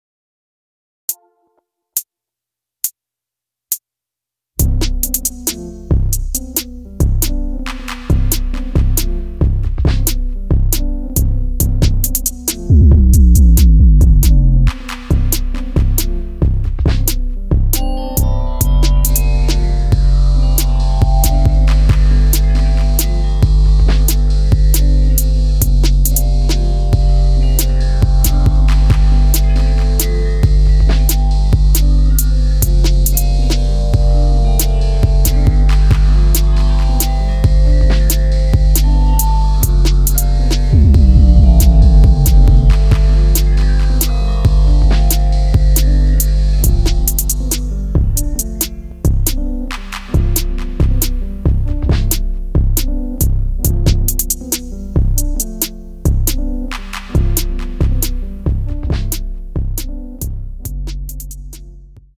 Herbert Hoover vs Wyoming East - High School Football
I like making little videos of the bursts of photos I take during a game.